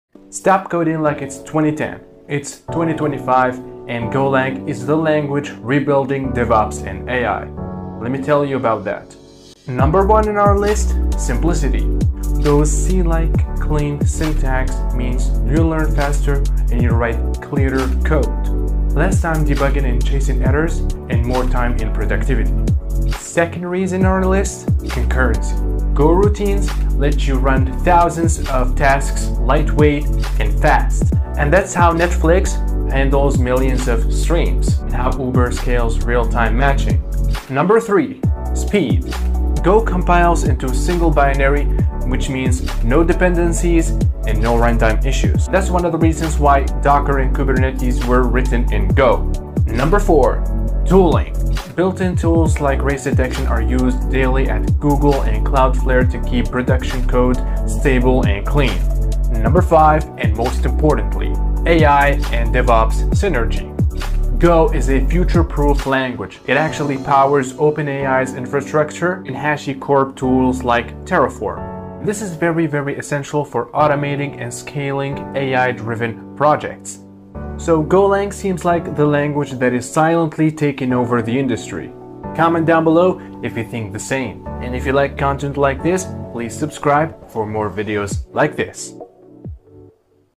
Golang In 2025
5 main reasons why you should learn GO

1 simplicity
2 concurrency
3 speed
4 tooling
5 Ai and Devops synergy